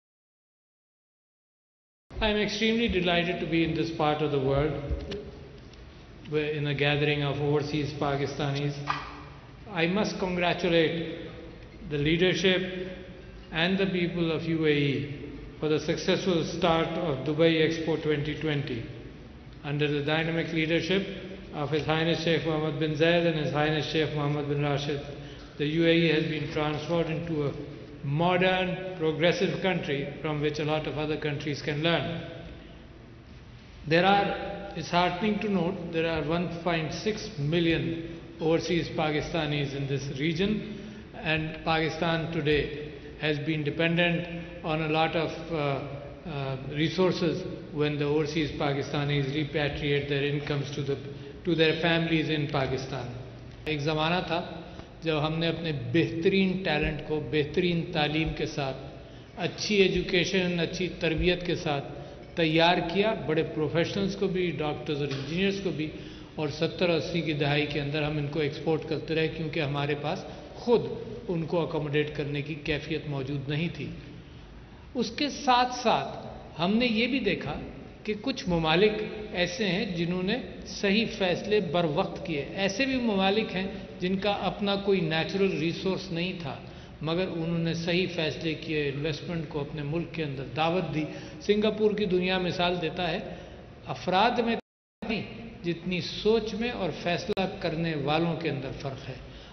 President invites expats to benefit from Pakistan’s secure investment policies
DUBAI, Oct 9 (APP): President Dr Arif Alvi on Saturday said a prosperous Pakistan was emerging in the global arena and urged overseas Pakistanis to benefit from the secure investment policies of the country.